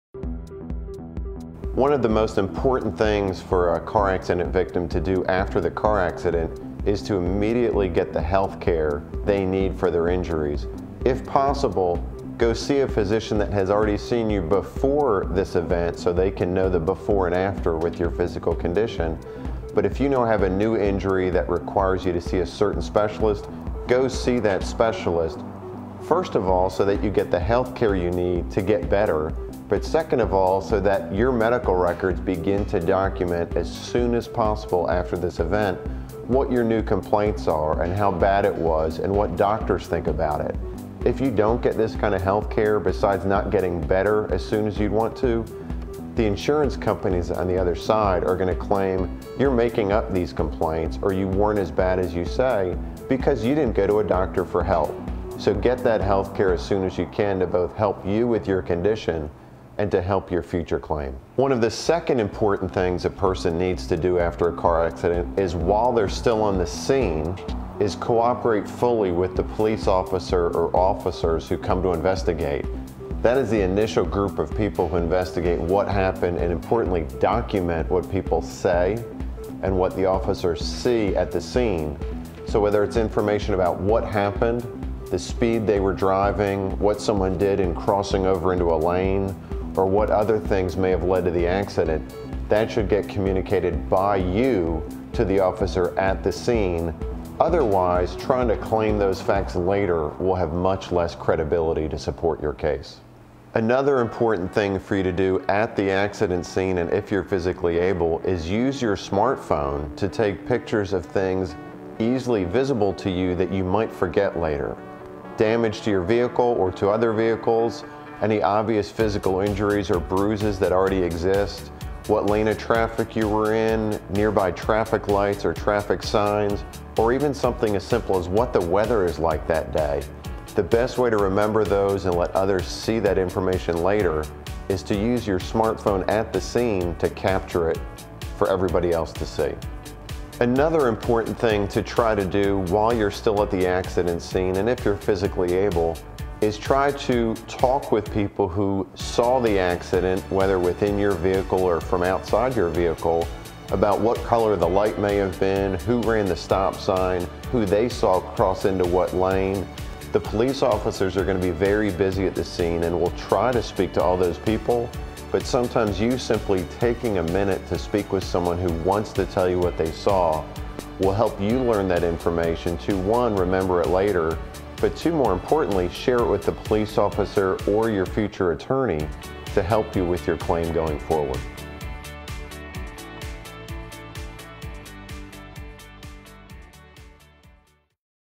Top Car Accident Lawyer Offers Tips to Car Wreck Victims - Cummings Manookian Trial Lawyers
- Brian Cummings of the Nashville based law firm of Cummings Manookian offers car accident victims sound advice and tip to consider in the event of an auto accident.

Cummings Manookian PLC
45 Music Square W